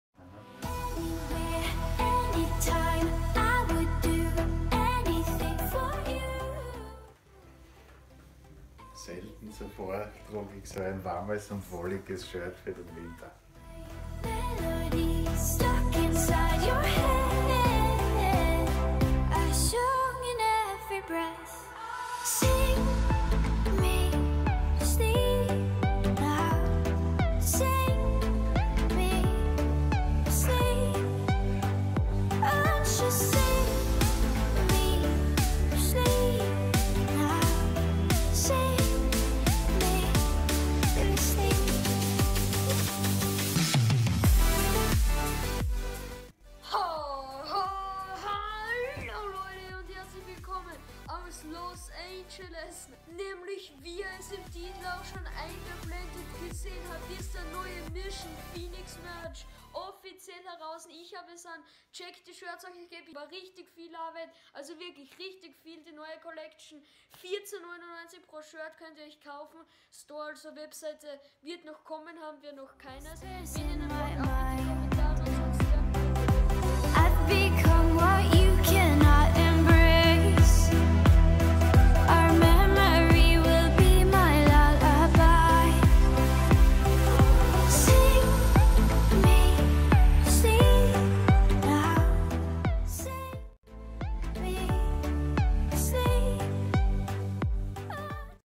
Mission Phoenix Merch - Out right NOW
Endlich ! Das neue Mission Phoenix Shirt ist nun endlich OFFIZIELL raus für nur 14,99 € also wenn ihr es kaufen möchtet oder Fragen habt ab in die Komments !